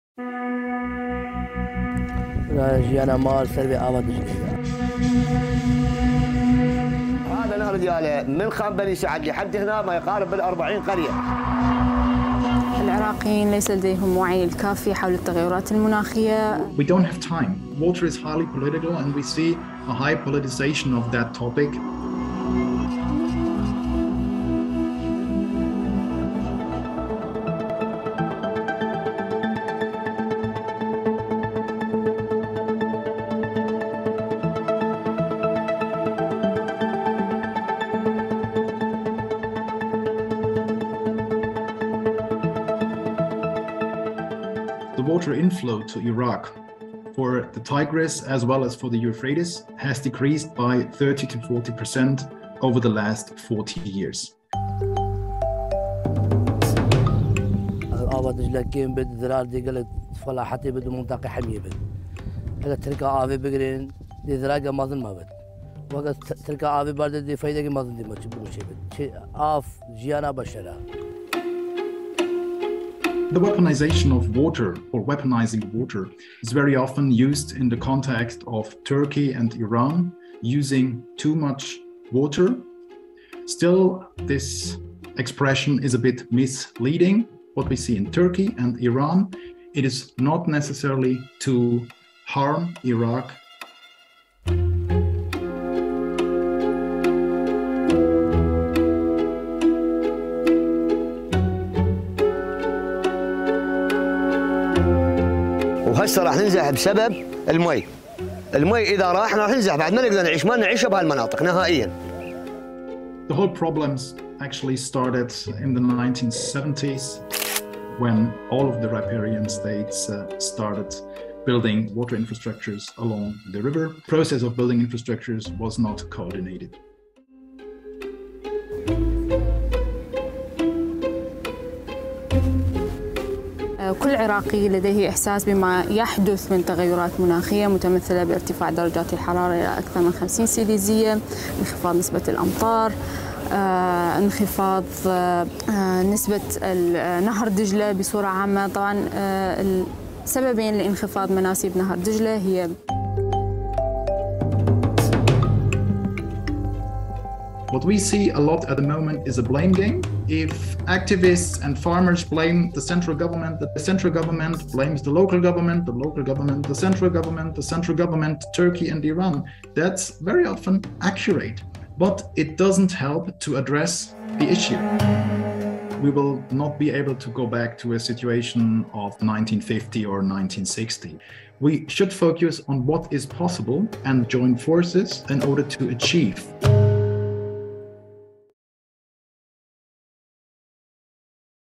দজলা নদী: তীব্র পানি সংকটে থাকা ইরাকিরা কেন ইরান ও তুরস্ককে দায়ী করছে?
"আমাদের জীবন-জীবিকা দজলা উপর নির্ভর করে," এ কথা বলেছেন উত্তর ইরাকের কৃষকরা। যারা তাদের ফসলের জন্য নদীর উপর নির্ভর করে।
বিশেষজ্ঞরা সতর্ক করেছেন যে এক সময়কার এই শক্তিশালী প্রমত্মা নদী যেটি বাইবেলের ইডেন গার্ডেনকে পানি দিয়েছিল এবং যে নদী মেসোপটেমিয়া সভ্যতার জন্ম দিতে সাহায্য করেছিল, এই প্রবাহমান নদী এখন শুকিয়ে যাচ্ছে।
শুধু জলবায়ু পরিবর্তনই এই পানির সংকটের মূল কারণ নয়, এর পেছনে জড়িয়ে আছে রাজনীতিও।
বিশেষজ্ঞরা বলেছেন, "পানি বিষয়টি অত্যন্ত রাজনৈতিক এবং তারা দজলা নদীর পানি বন্টনের ক্ষেত্রে বড় ধরণের রাজনীতিকরণ দেখতে পাচ্ছেন।"